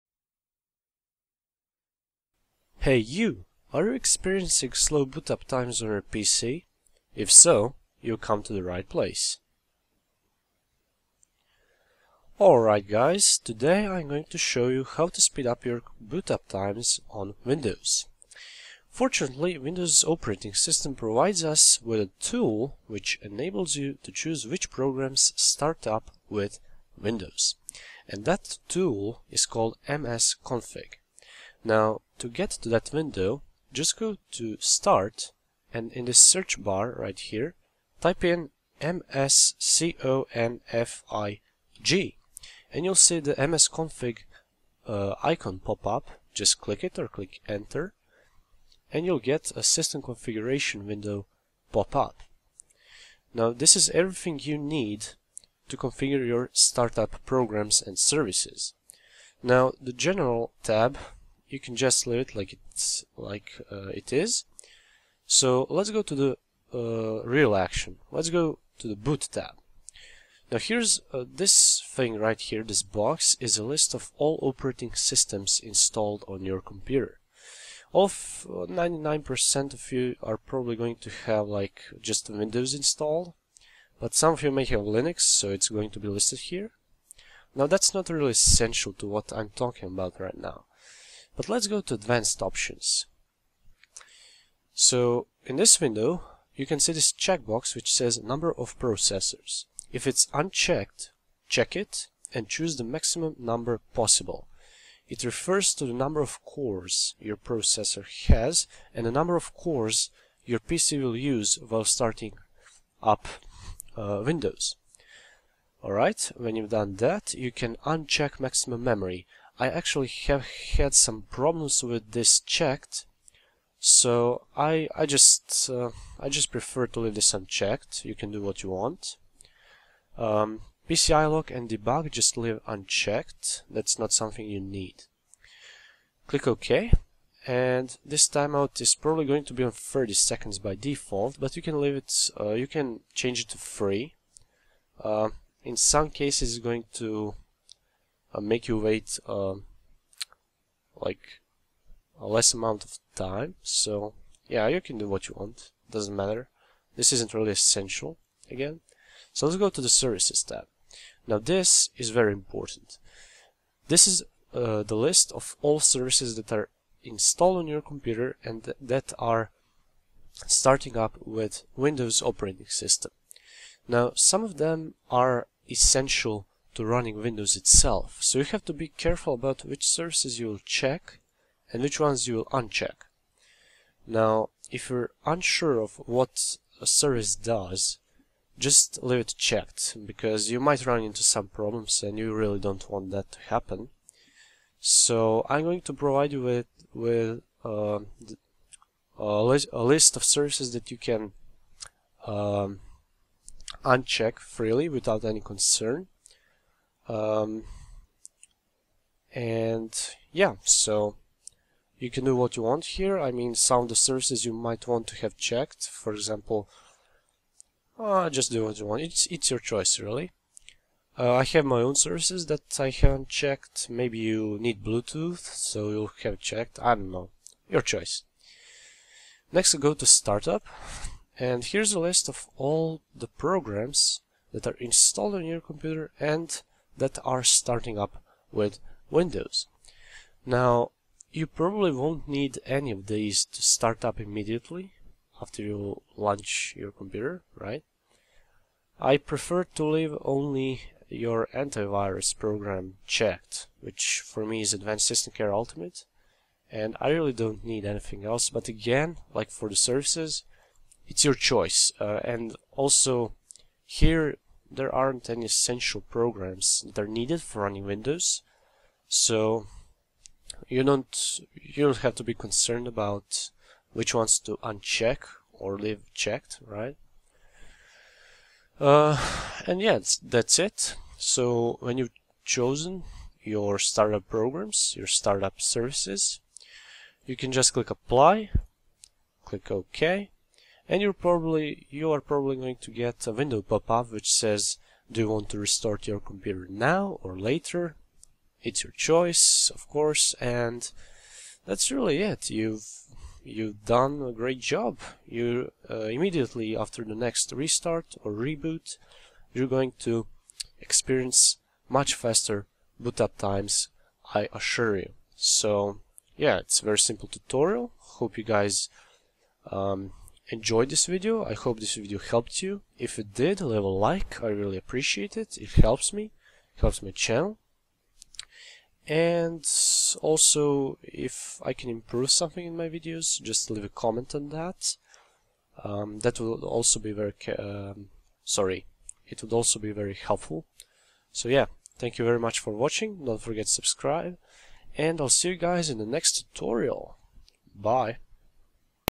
MSConfig - Improve your boot up speed
Hey guys. :)
A lot of people sometimes wander how to speed up their computer, especially the boot part where people often have to wait 10 minutes before they start using the PC because of a thousand programs loading... I'm showing you how to prevent and stop that with a very useful tool integrated into Windows operating system ;)

This method will work on Windows XP/Vista/7/8/8.1 and Server Editions of Windows. For earlier versions, like Windows 2000/98/95 you'll need to download an alternative that you can get here:

Some services that you can disable in "Services" tab without any problems:

ActiveX Installer
Adaptive Brightness
Adobe Flash Player Update Service
ASP.Net State Service
Bluetooth Support Service (if your machine doesn't support Bluetooth)
Fax (if you're not using a fax)
Homegroup Listener
Homegroup Provider
Microsoft .NET Framework NGEN
Microsoft Sharepoint Workspace Audit Service
Office Software Protection Platform
Parental Controls
Print Spooler (if you're not using a printer)
Tablet PC Input Service
WinHTTP WebProxy Auto Discovery Service
Workstation
WWAN Autoconfig
WLAN or Wired Autoconfig (depending on your type of connection)